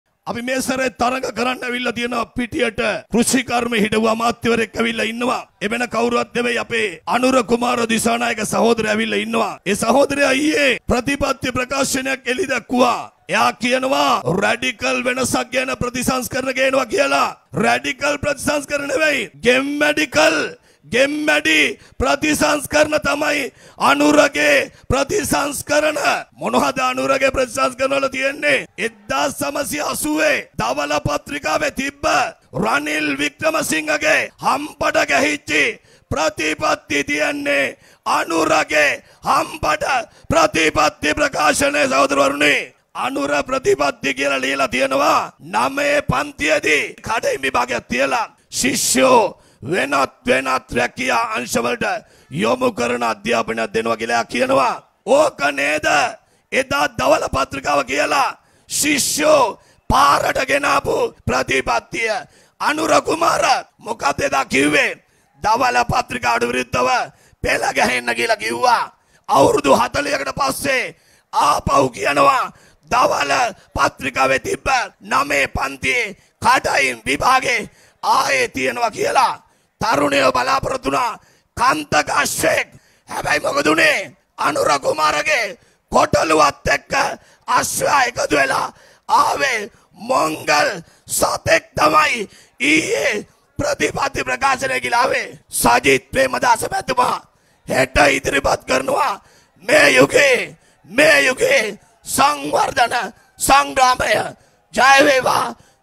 අනුරගේ-රනිල්ගේ හම්පඩ ප්‍රතිපත්ති ගැන යාපාබණ්ඩාර කියයි.
සමගි ජන සන්ධානය අම්පාර ජනරැළිය